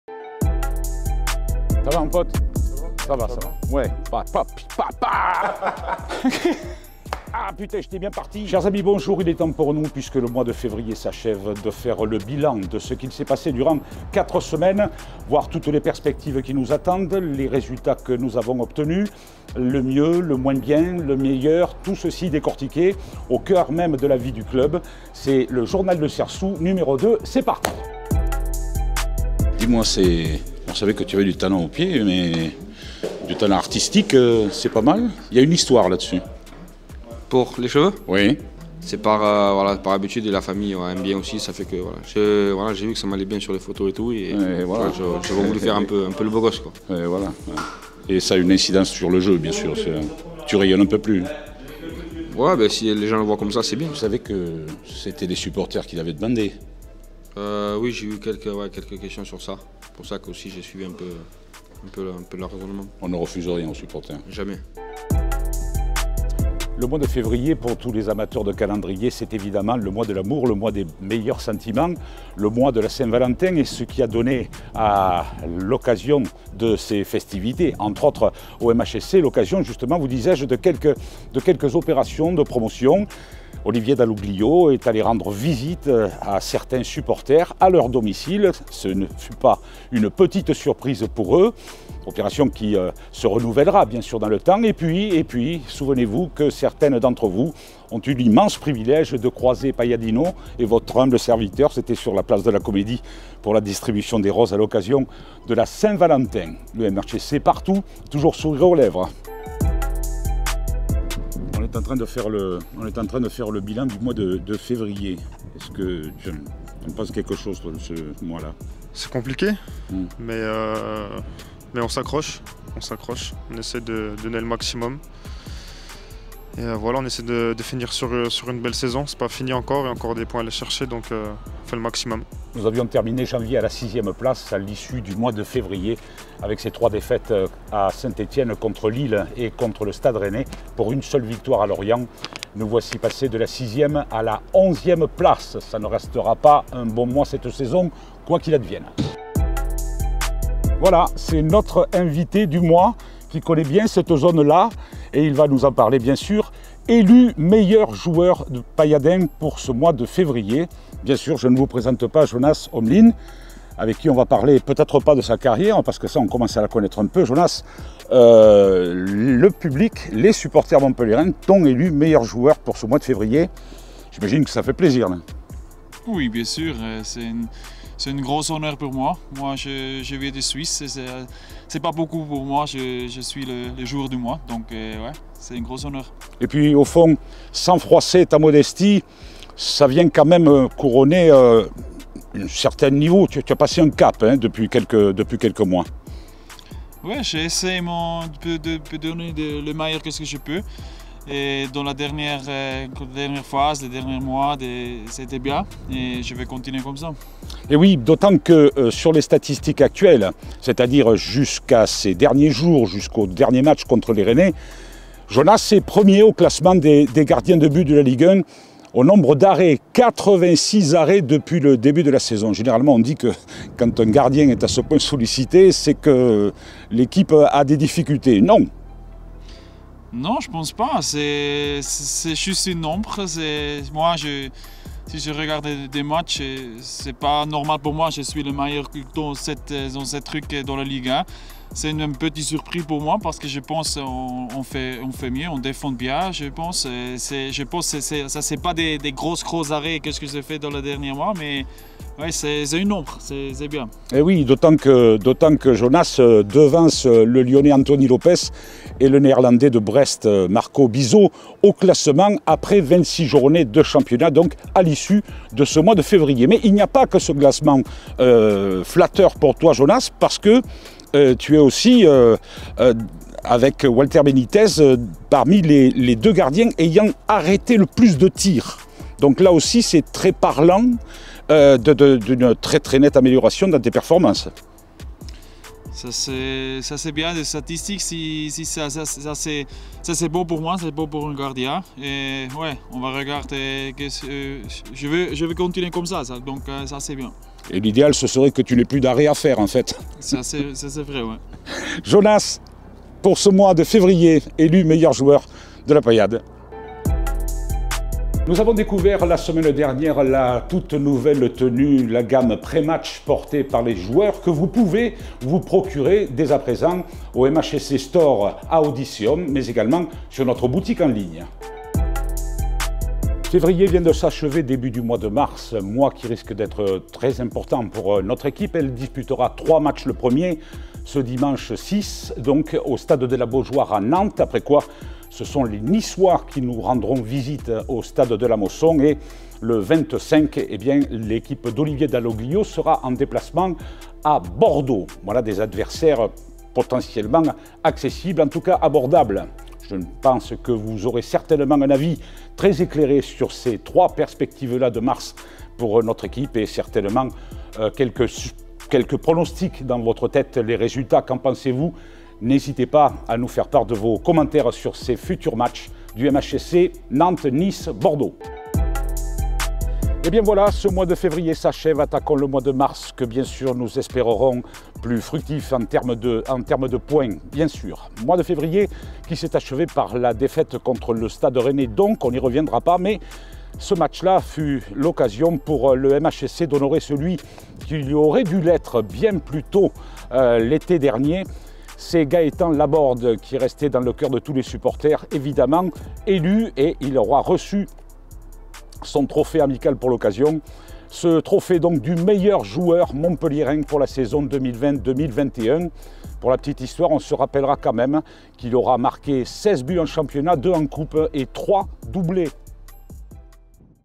Journal de Sersou février JDS 2
0:00 intro
0:28 la story de sersou
1:05 info club
1:48 bilan du mois
2:29 invité du mois
5:06 nouveauté
5:23 mois de mars
6:07 gaëtan laborde remise